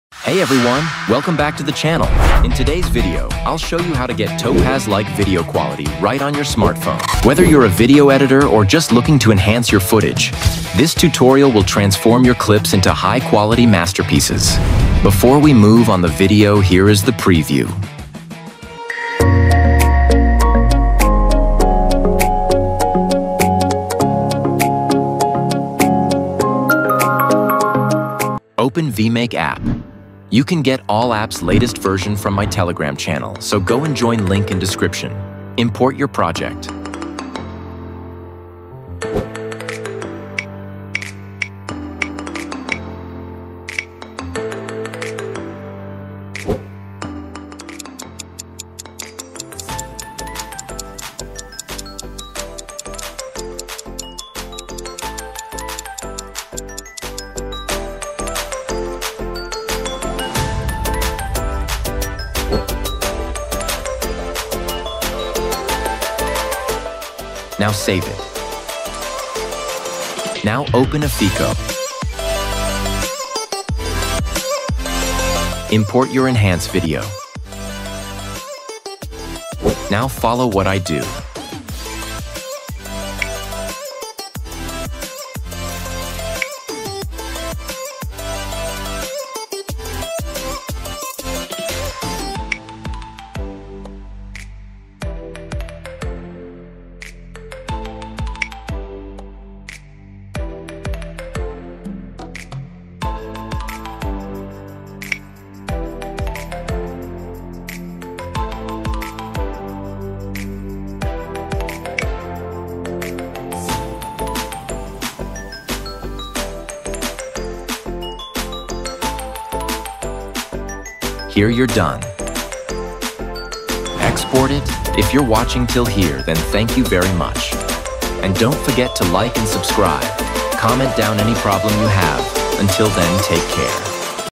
Create Topaz-Like Quality on Mobile | Enhance Quality
In this tutorial, I’ll show you how to achieve Topaz-like professional video quality on your mobile device! Using the right tools and techniques, you can create stunning edits that look like they were made with advanced software. This video is perfect for anyone looking to enhance their video quality using Alight Motion and mobile editing apps.

🔹 What’s covered in this video:

• Tips to enhance video sharpness and clarity

• How to achieve smooth frame interpolation

• Best settings for high-quality renders in Alight Motion

• Mobile-friendly tools for professional-quality edits

📥 Download free presets and resources here:
📱 Join my Telegram channel for exclusive content and free downloads: